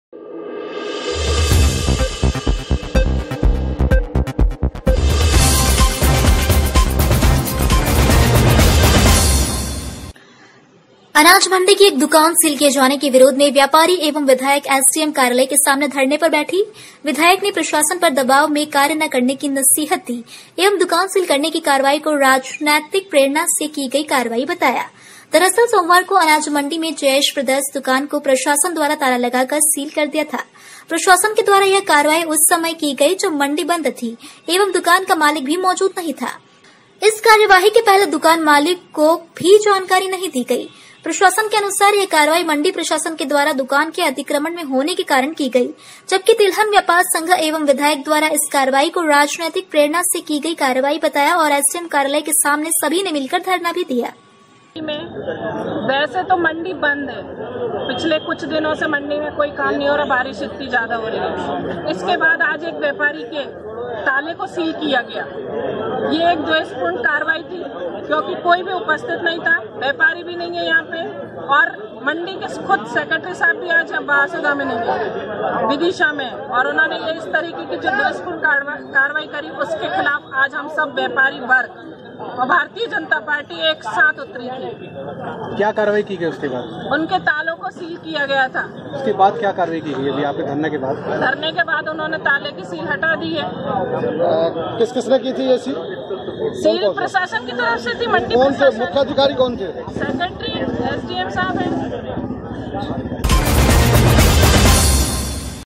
दुकान सील किए जाने के विरोध में व्यापारी एवं विधायक एसडीएम कार्यालय के सामने धरने पर बैठी
अनाज मंडी की एक दुकान सील किए जाने के विरोध में व्यापारी एवं विधायक एसडीएम कार्यालय के सामने धरने पर बैठी | विधायक ने प्रशासन पर दबाव में कार्य ना करने की नसीहत दी एवं दुकान सील करने की कार्यवाही को राजनैतिक प्रेरणा से की गई कार्यवाही बताया! दरअसल सोमवार को अनाज मंडी में जयेश ब्रदर्स दुकान को प्रशासन द्वारा ताला लगाकर सील कर दिया था! प्रशासन के द्वारा यह कार्यवाही उस समय पर की गई जब मंडी बंद थी एवं दुकान का मालिक भी मौजूद नहीं था! इस कार्यवाही के पहले दुकान मालिक दुकान मालिक को कोई भी जानकारी नहीं दी गई | प्रशासन  के अनुसार  यह कार्यवाही मंडी प्रशासन के द्वारा  दुकान के अतिक्रमण में होने के कारण की गई | जबकि तिलहन व्यापार संघ एवं विधायक द्वारा इस कार्यवाही को राजनीतिक  प्रेरणा से की गई कार्यवाही बताया और एसडीएम कार्यालय के सामने सभी ने मिलकर धरना भी दिया!